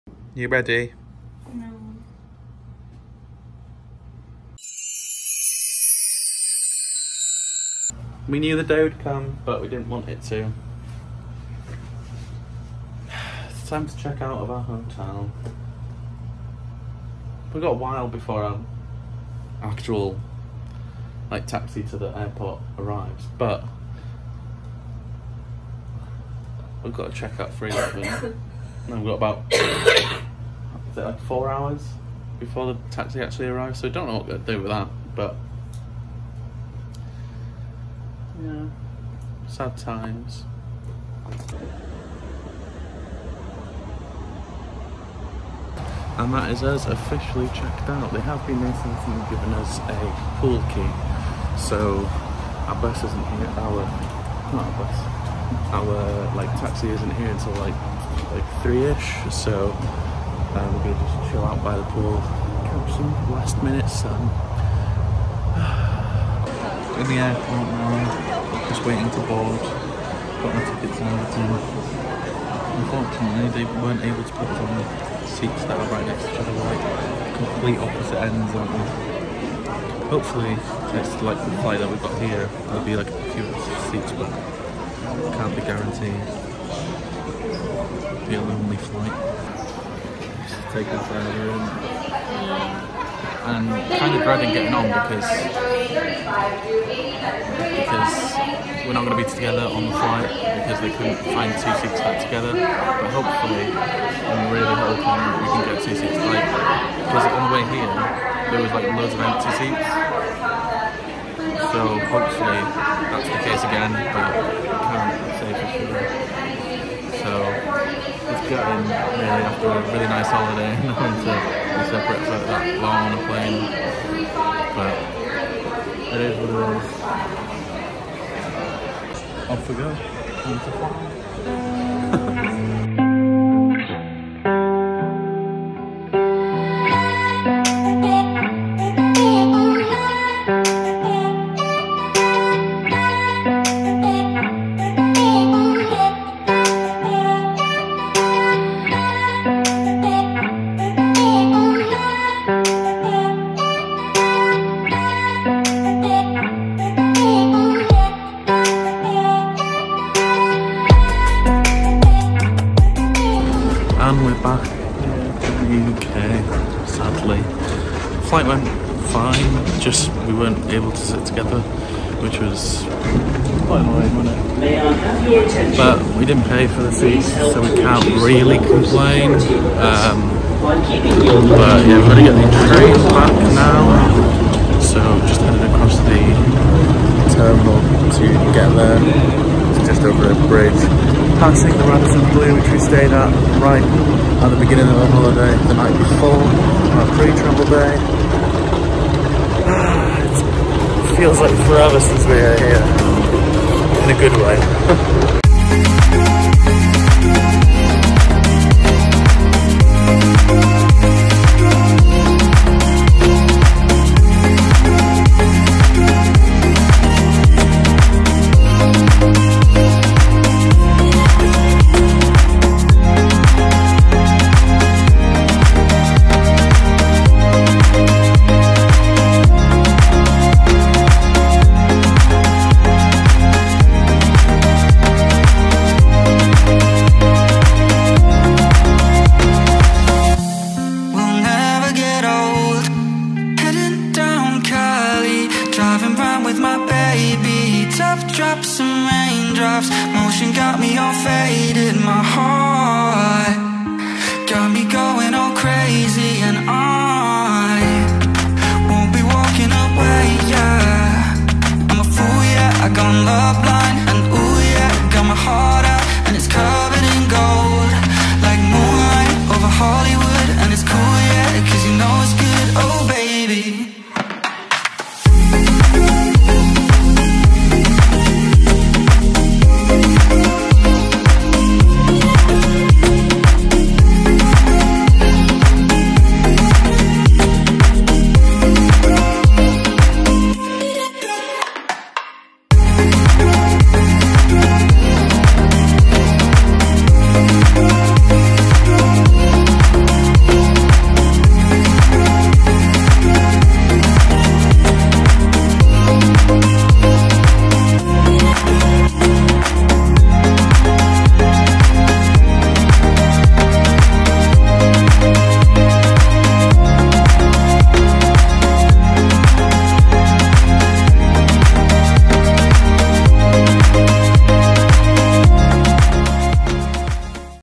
Travel Day Home Orlando To Manchester | Florida November 2019 | Travelling Magical
Join us (Ashley & Emily) for fourteen days in Orlando, Florida!

It's travel home day of our Orlando vlog series. We travel from Orlando International Airport to Manchester Airport after an amazing 14 day trip to Orlando where we got engaged! We've had some magical memories from this trip and wouldn't change it for the world, thank you to everyone who's followed along on our first major vlog series, safe to say that there's plenty more where that came from, we have so many exciting things to share with you so subscribe to keep up to date with all our future magical adventures.

Music Credits 🎤

💫  SOCIAL MEDIA  💫